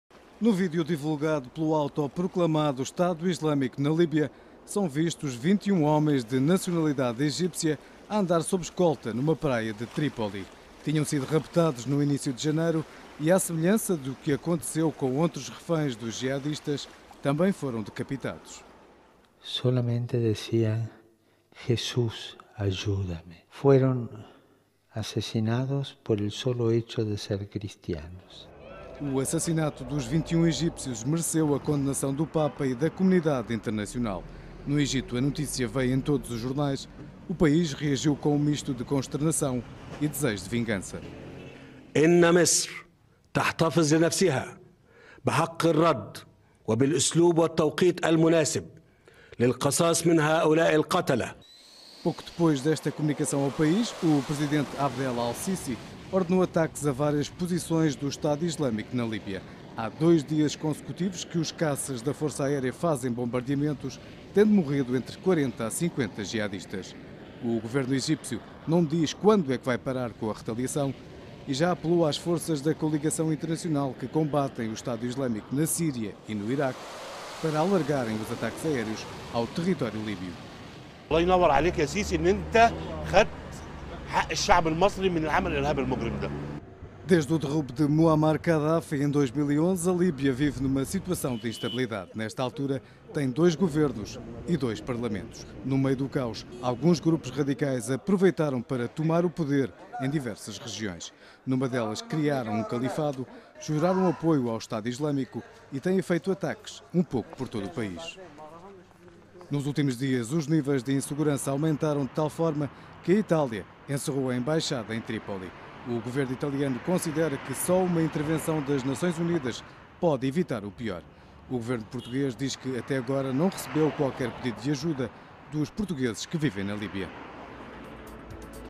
Papa Francisco condenou execução de 21 cristão coptas na Líbia
O Egipto bombardeou pela segunda vez diversos alvos do grupo Estado Islâmico na Líbia. É a resposta armada à divulgação de um vídeo em que é mostrada a decapitação de 21 cristãos egípcios. Mais decapitações feitas pelos jihadistas. .